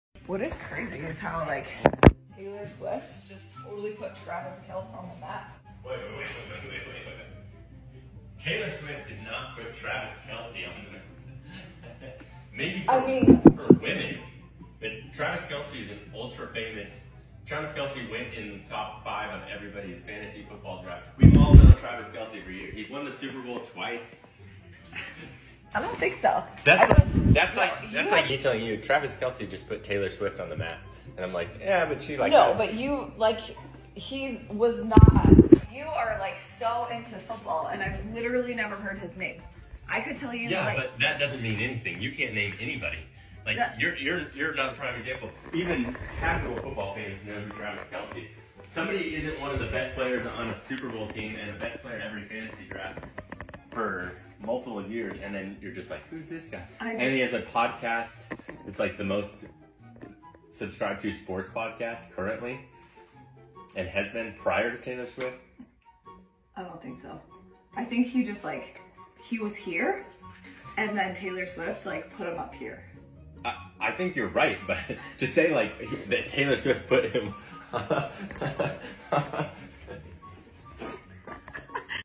Telling My Husband That Taylor Swift Put Travis Kelce On the Map
PLEASE tell your partner that "it's crazy how Taylor Swift just put Travis Kelce on the map" and watch him lose his mind.

The LECTURE I got! 🤣🤣🤣🤣🤣